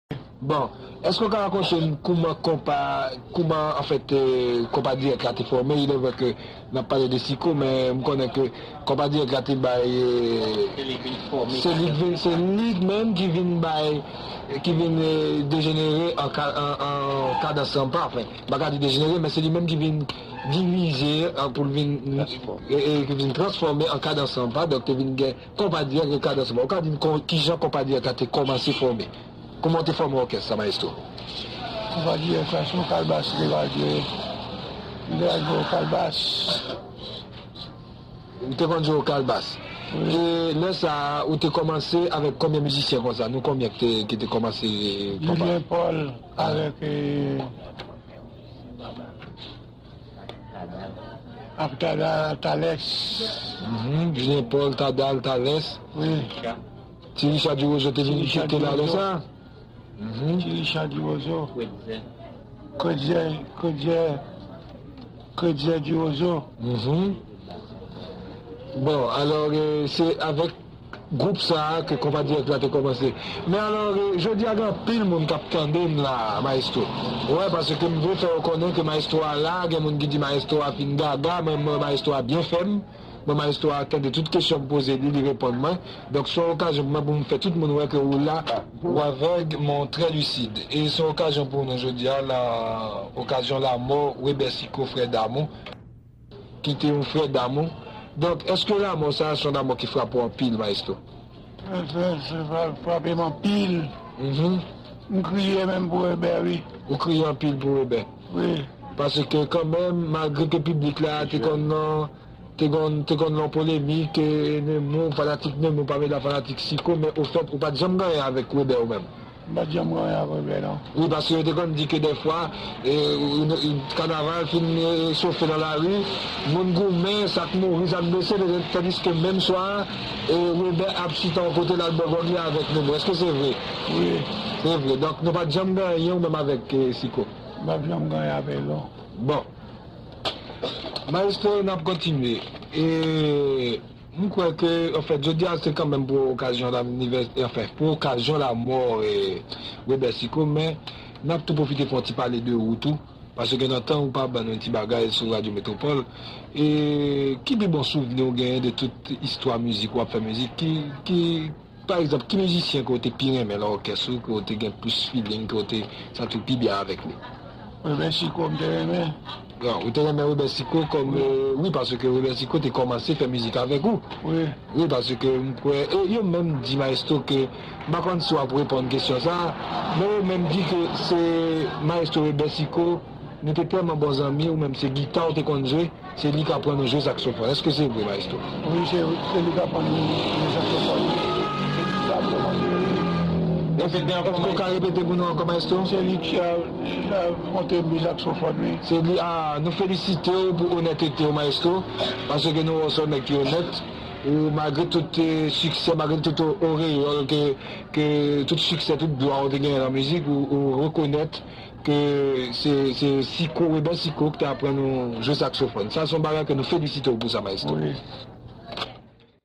Interview de Nemours Jean Baptiste sur Webert Sicot par Joe Damas
NEMOURS JEAN BAPTISTE KAP Pale de WEBERT SICOT ak JOE DAMAS.
Yon ekstrè de youn entrevu ke Joe Damas te fe ak Nemours (Kreyate COMPAS DIRECT) pou lanmò Sicot an Fev 85. Radio Metropole te pase nan yon jounen ke yo te òganize pou selebre lavi de Nemours Jn Baptiste an Mai 85. Nou ka konstate gwo travay ke Joe damas tap fe sou mizik ayisyen e respè ki te genyen ant de GWO MAESTWO sa yo